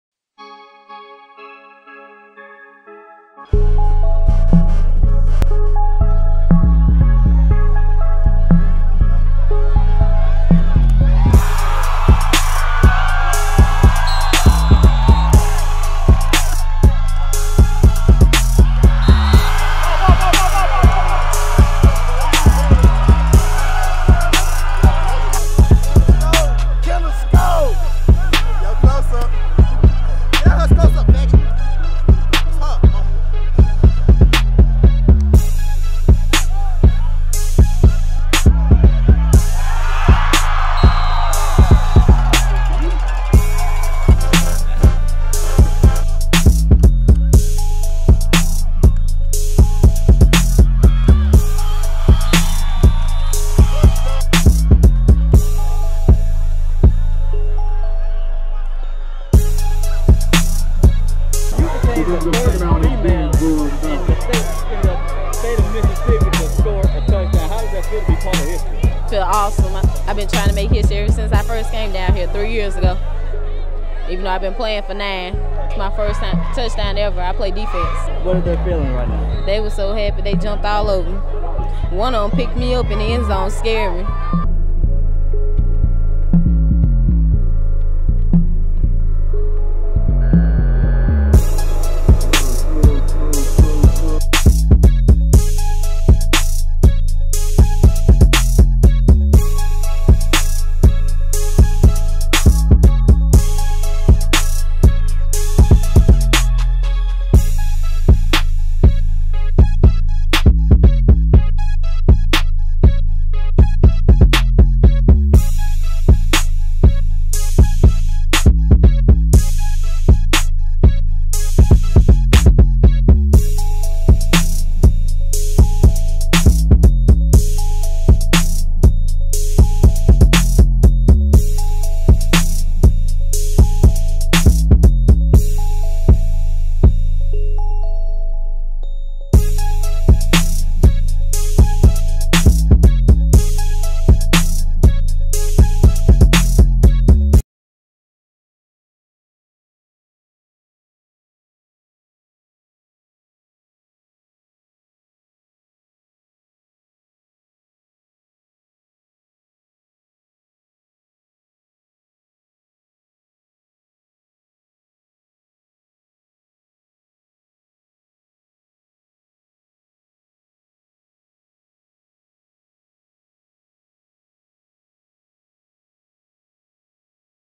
Laurel vs Wingfield/South Jones vs Natchez football highlights
Highlights of Laurel High School vs Wingfield High/South Jones High School and Natchez High School.